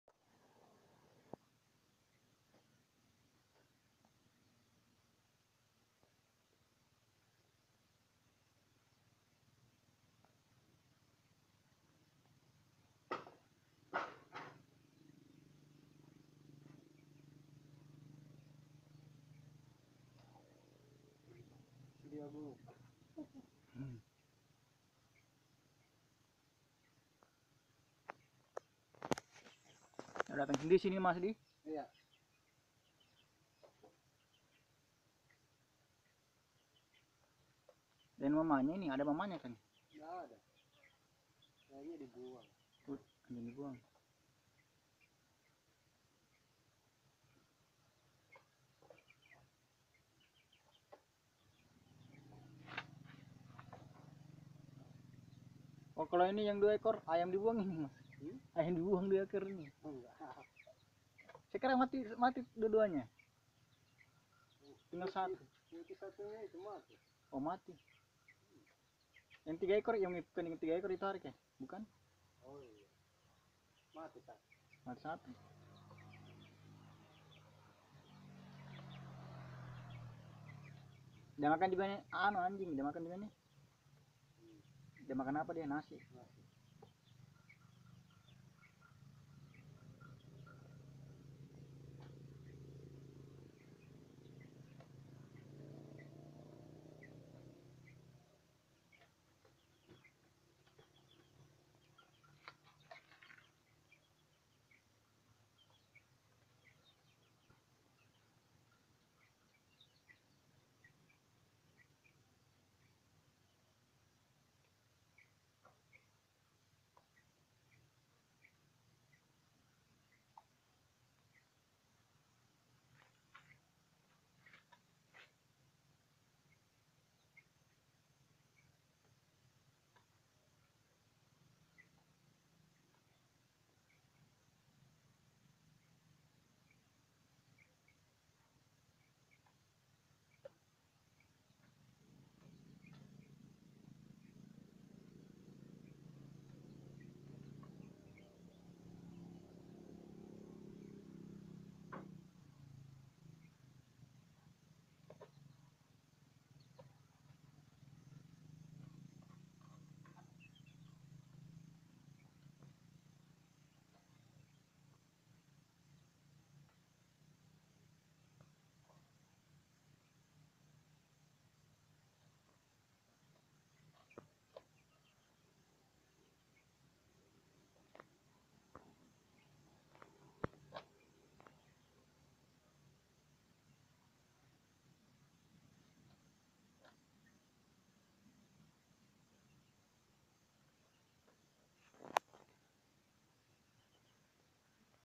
Viral, kasihan anak anjing di buang majikannya
Pikiran saya pun langsung menebak kalau anjing ini sengaja “dibuang” oleh pemilik lamanya karena (mungkin) dia tidak mau memeliharanya lagi.